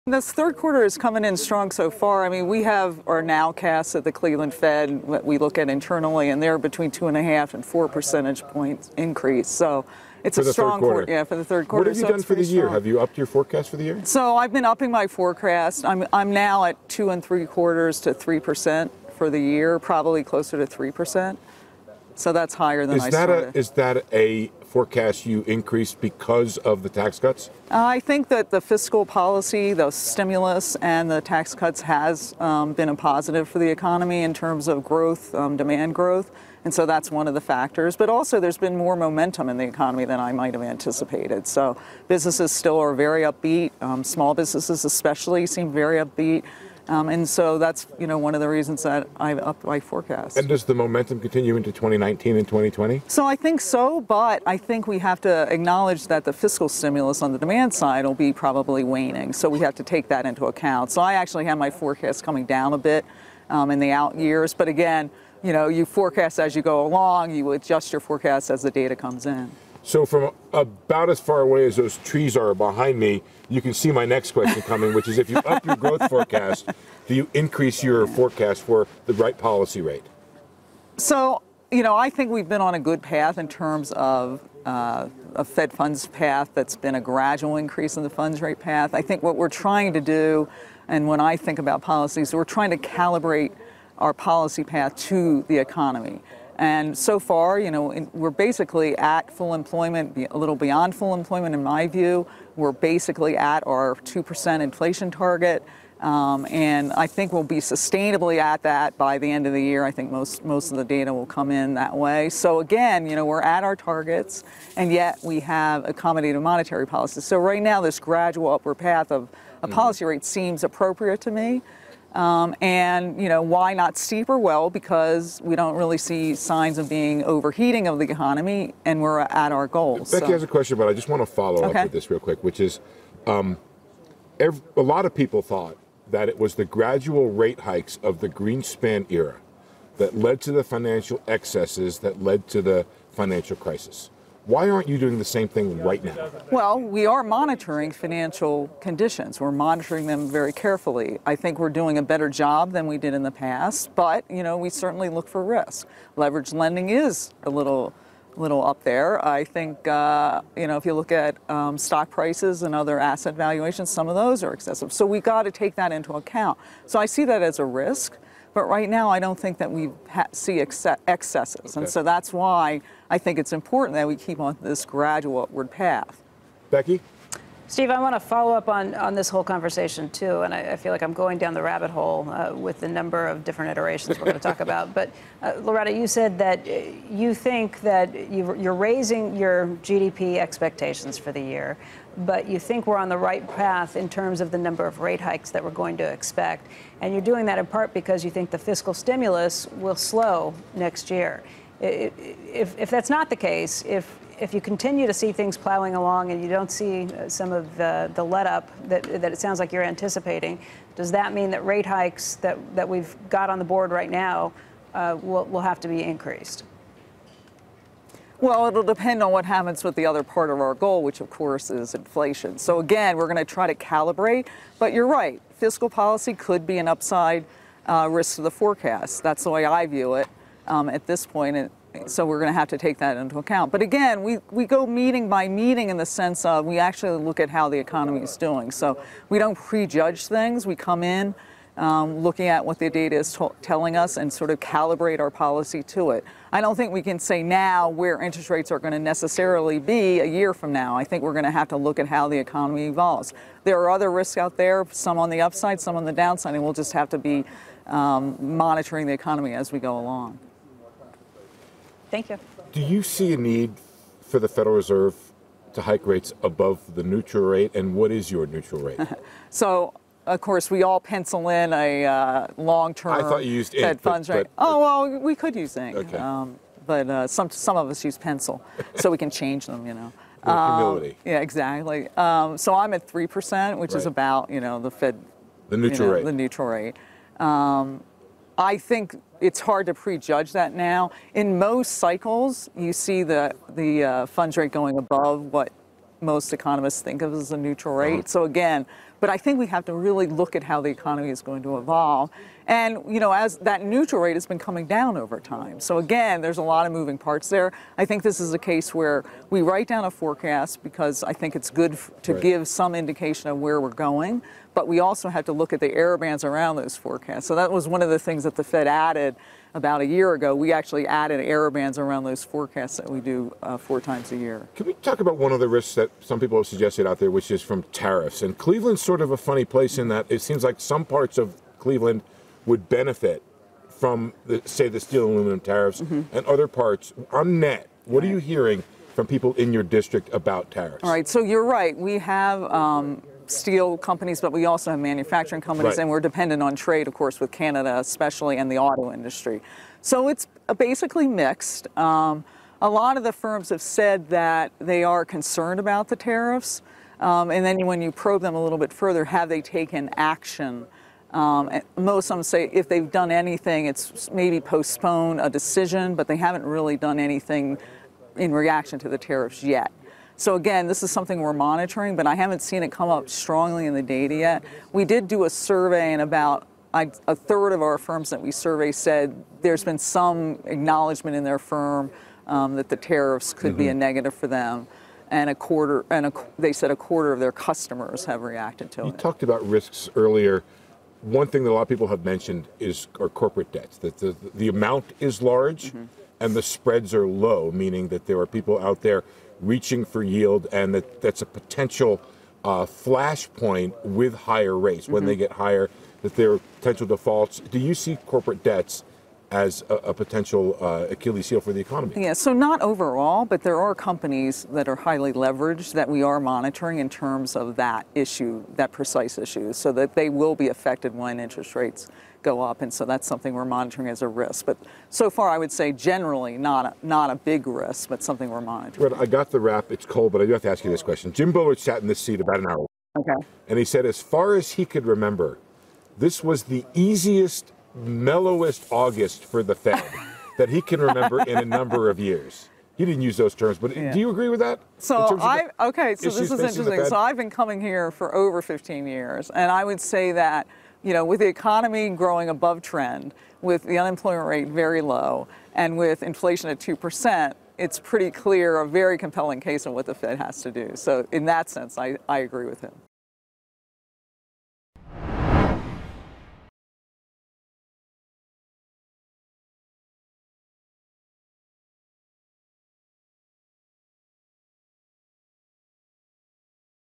Cleveland Fed president on the US economy, risks and tariffs
Cleveland Fed President Loretta Mester joins CNBC's Steve Liesman to discuss the U.S. economy, rate hikes and the risks the Fed is considering.